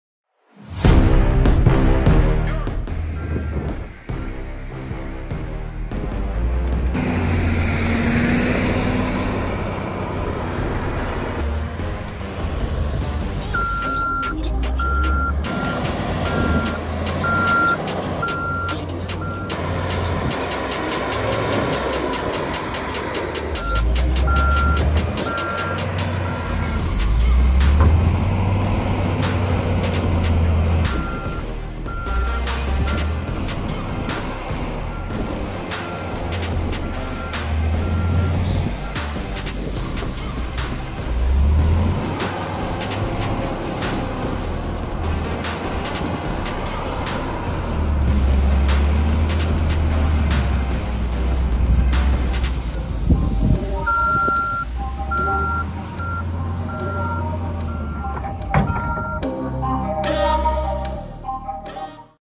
Volvo L120E Trabajando con estiércol I Tractores Horticampo
Mas informacion.

📍Camino De Cortijo San Luciano, Sn
     04720 La Puebla de Vicar - Almería.